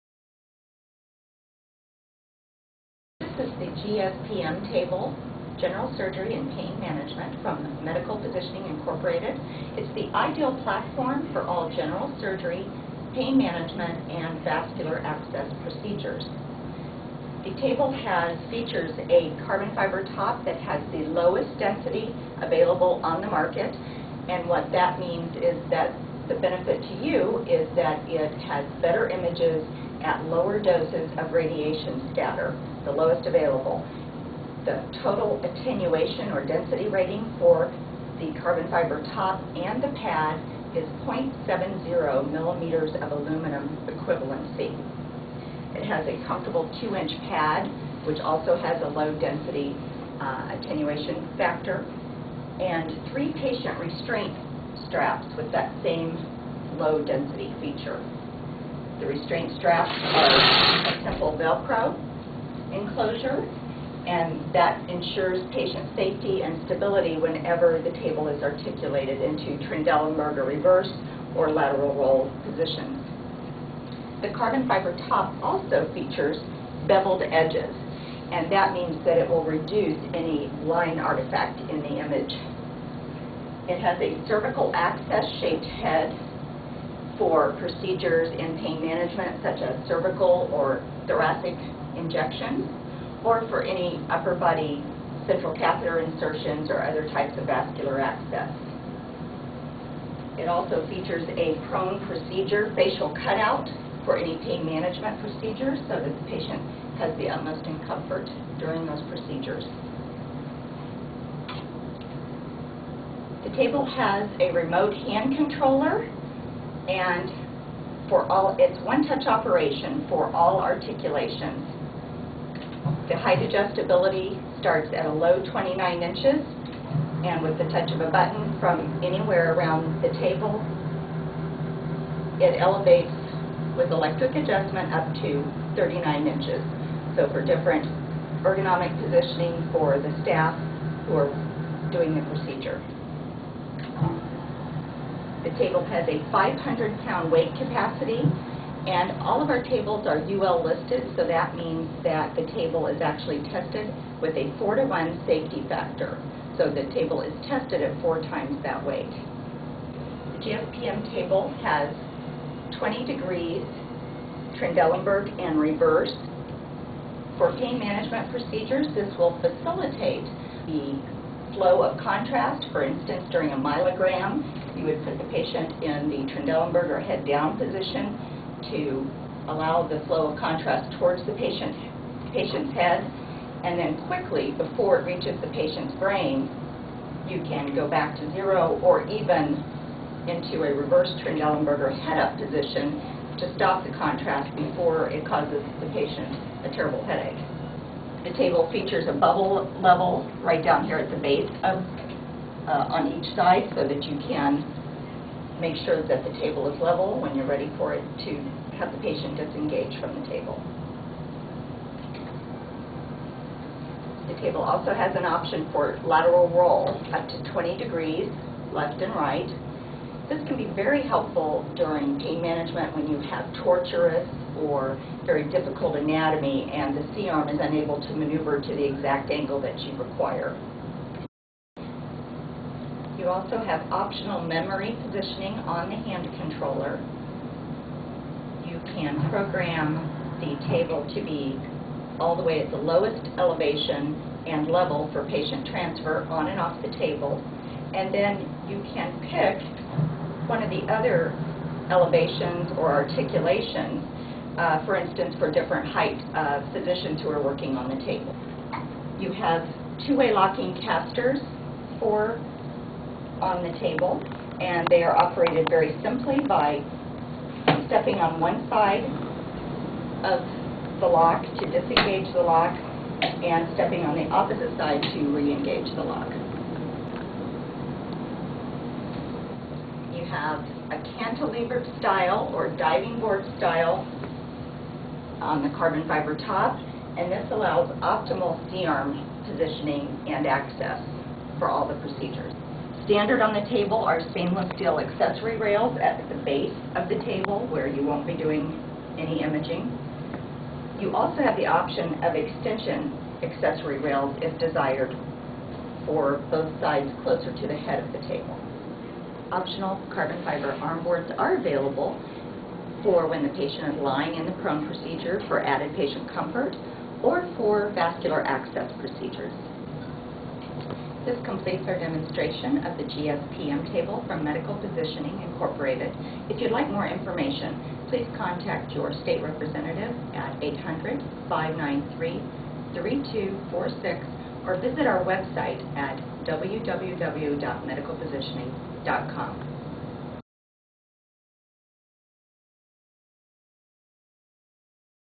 GSPM Table Demo from Medical Positioning
The GSPM™ Table is the ideal platform for pain management procedures, such as vertebroplasty or kyphoplasty, that require oblique imaging. The GSPM™ Table can also help expedite procedure time by properly positioning the patient to facilitate contrast transfer during procedures such as myelograms. Its low-attenuation table top provides superior images and ensures the lowest levels of radiation exposure to patients and clinicians.